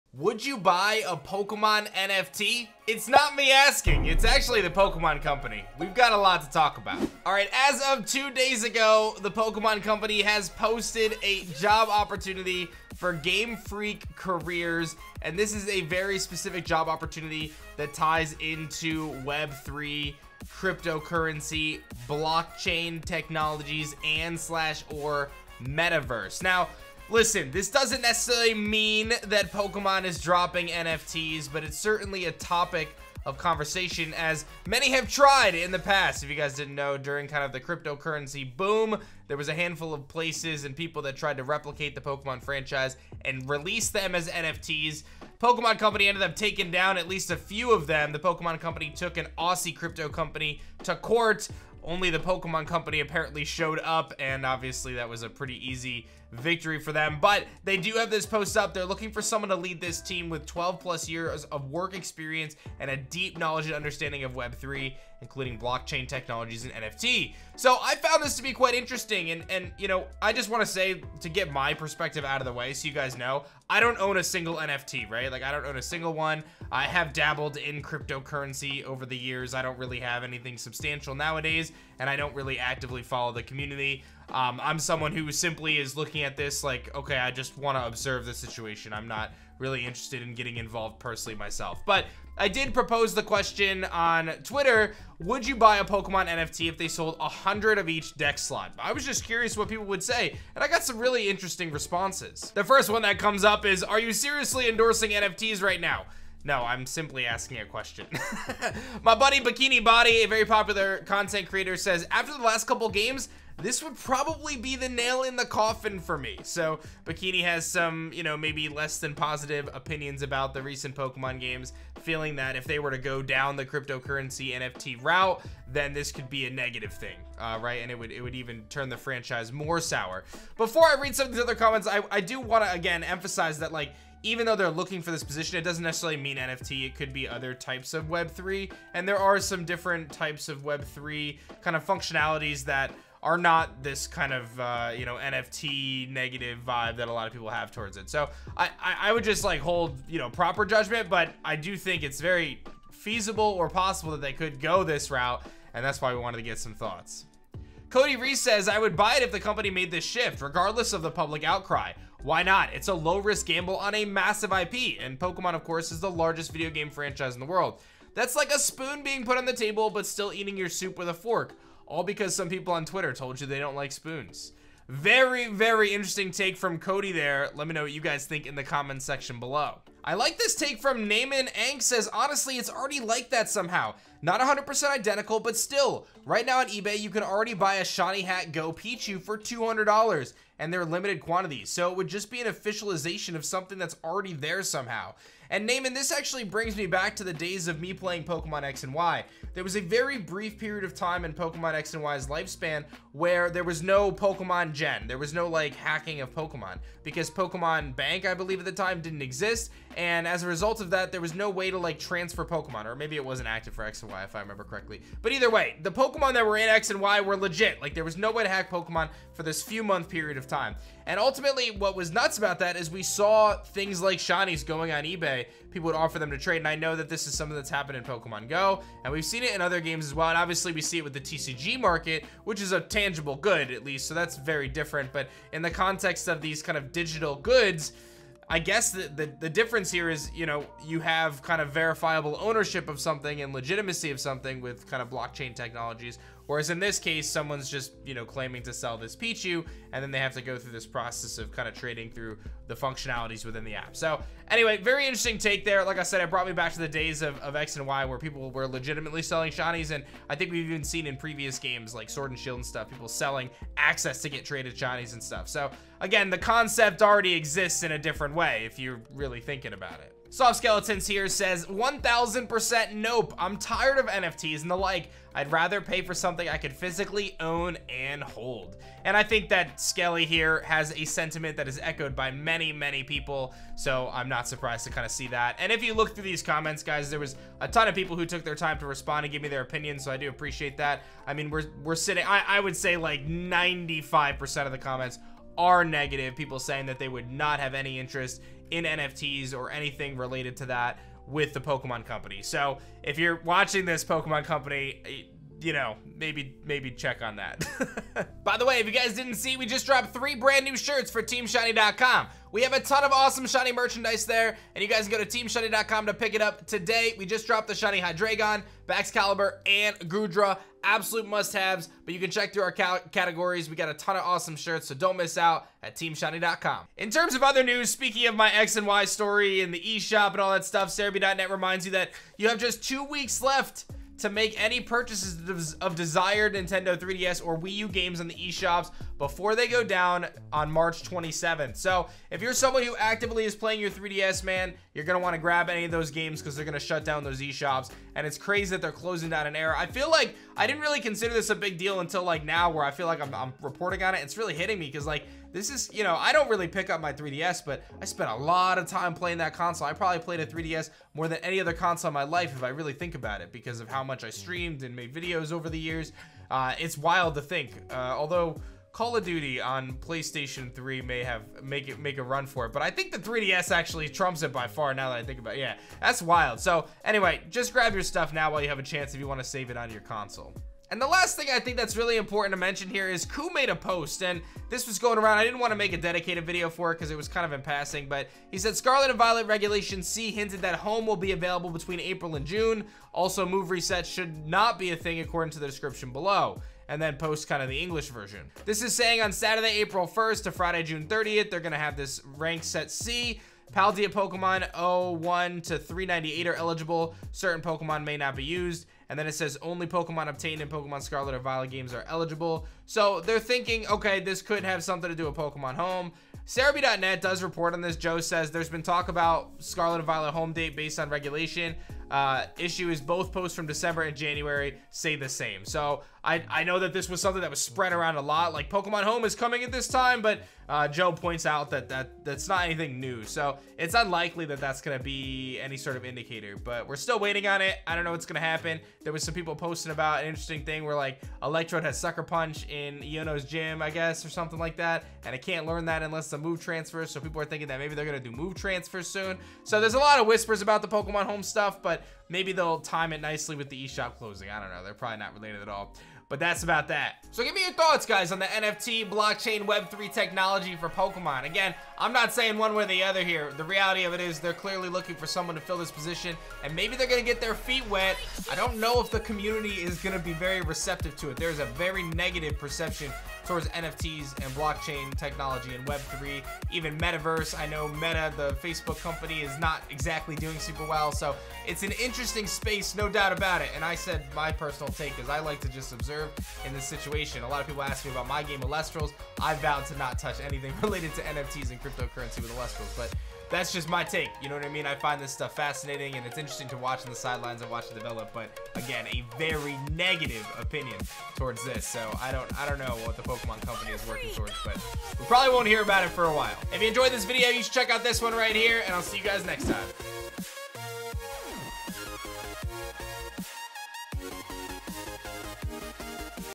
Pokemon NFTS...? Is this the END?
Talking about the latest job posting for Pokemon indicating theyre hiring a web3coder for focus in developing NFTs potentially, what is your thoughts?
Be sure to check out for epic swag!

Update for Pokemon and their new web3 job